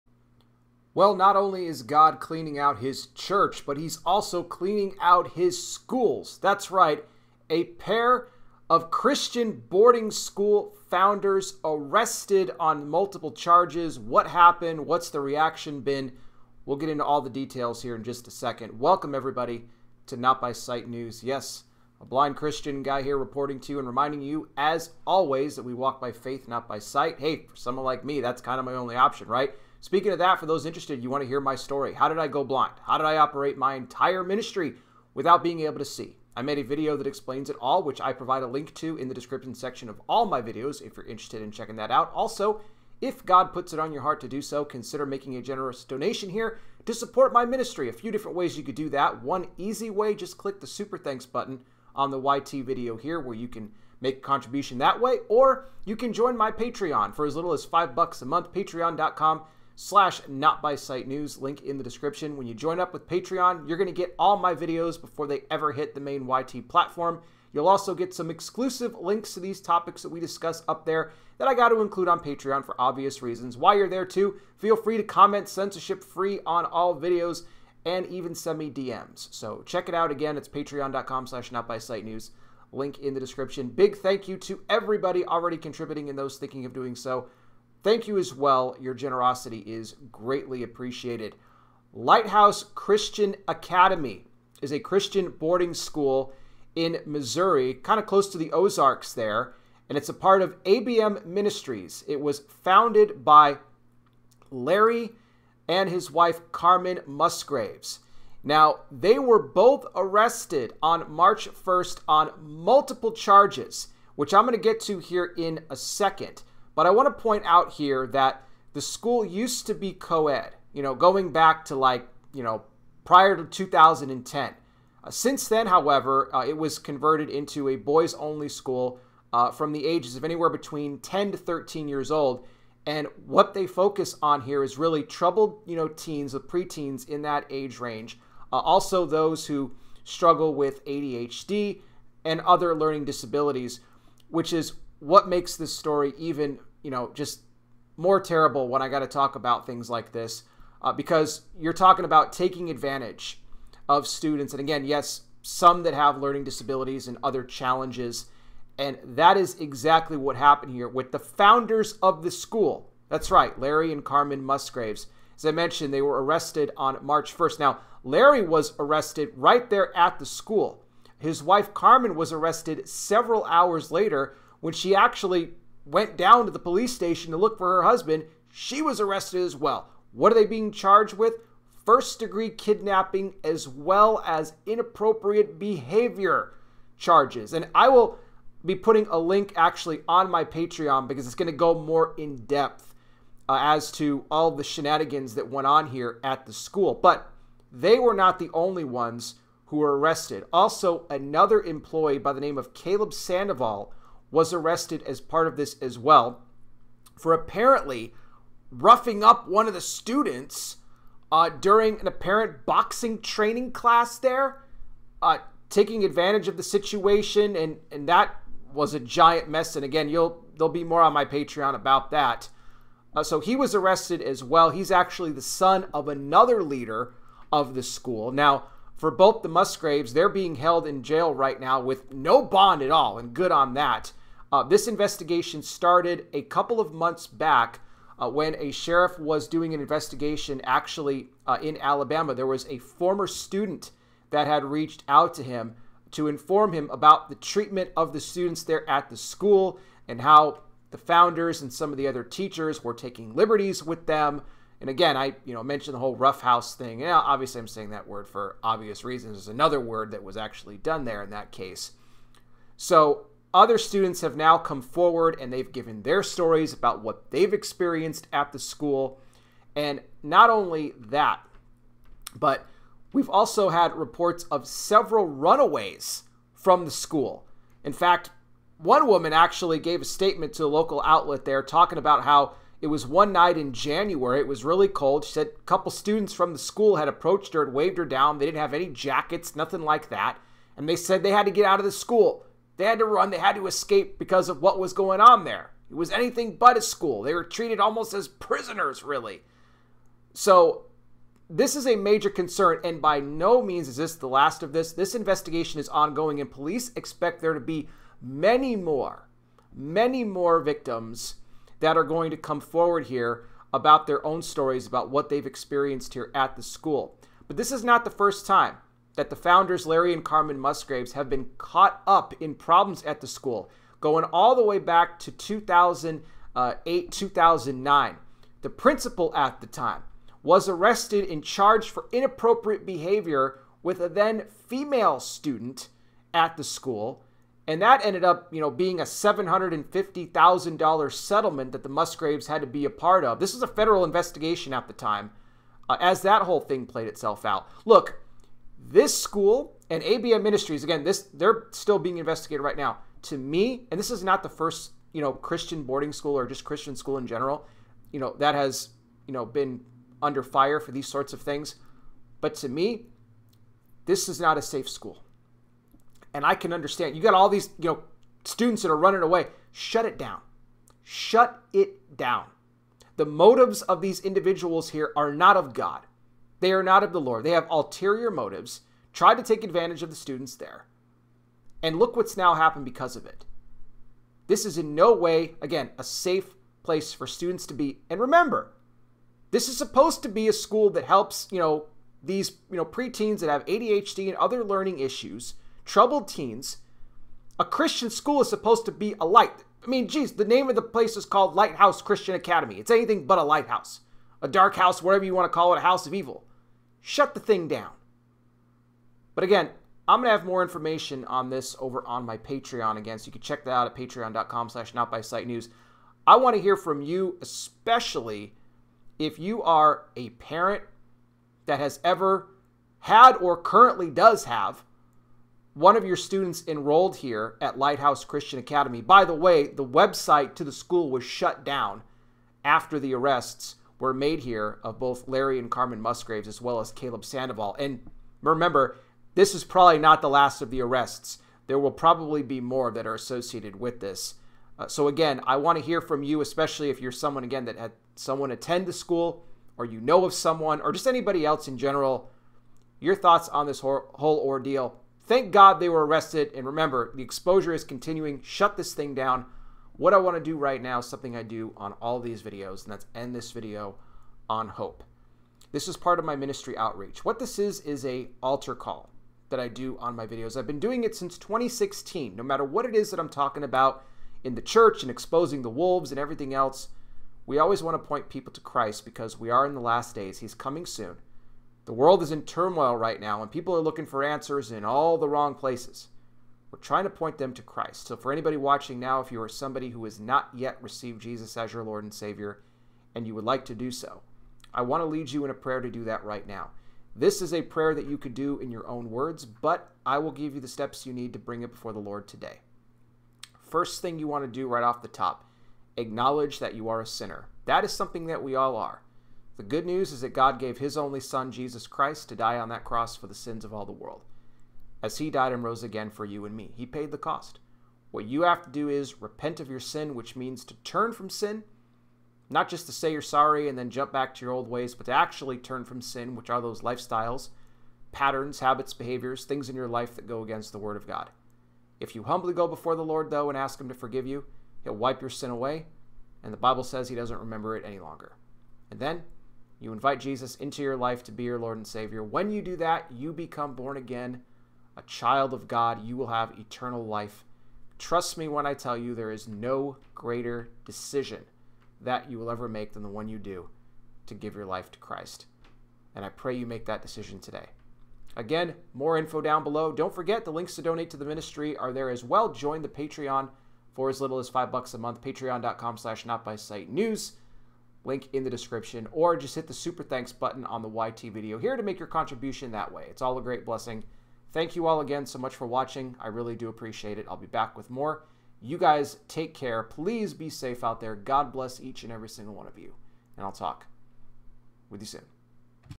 Lighthouse Christian Academy Founders Larry & Carmen Musgraves Arrested SHOCKING DETAILS 15 Mar 2024
MY STORY ON GOING BLIND & HOW I OPERATE MY VIDEO MINISTRY